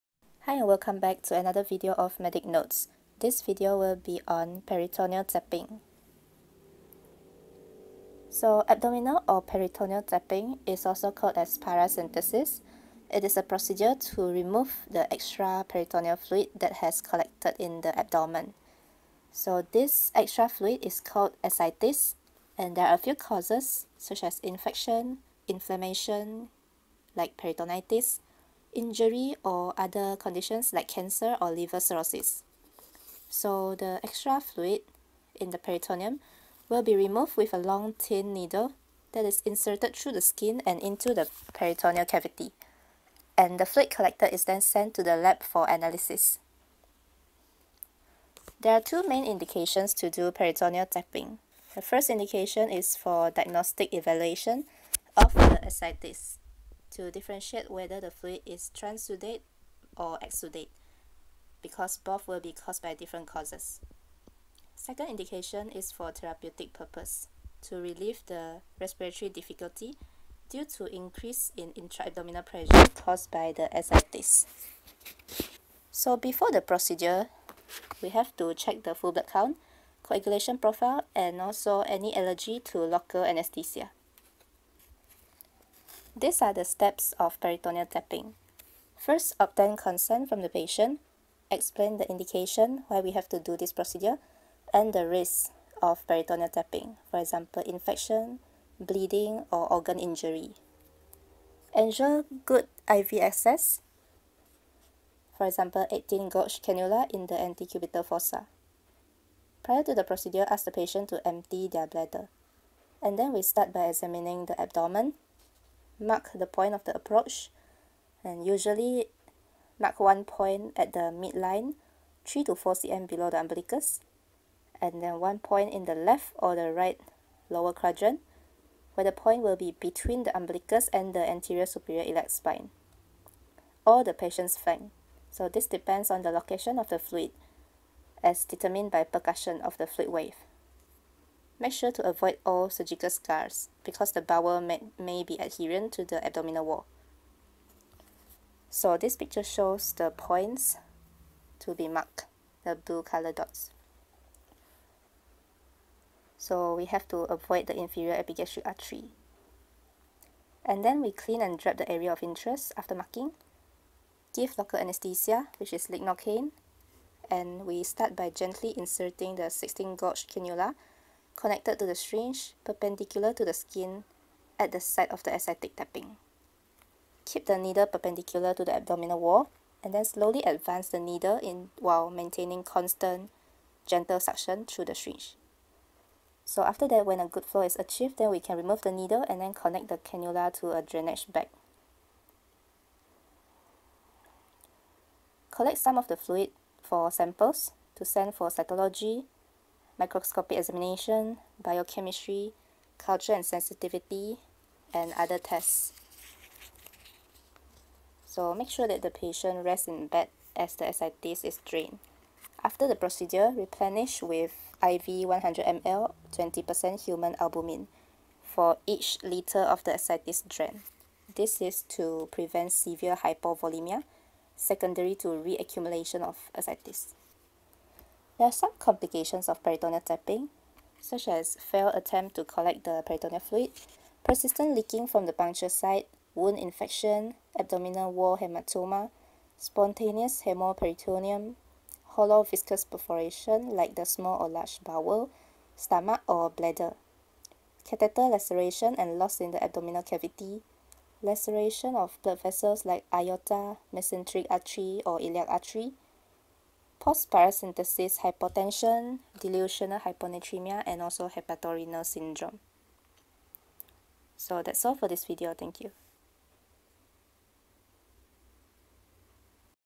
[Houseman preparation procedures] Peritoneal tapping/ Paracentesis - procedural steps
This is a series of clinical procedures for housemanship preparation. This video is on peritoneal tapping aka paracentesis, it includes the definition, things to do before procedure, procedural steps and complications.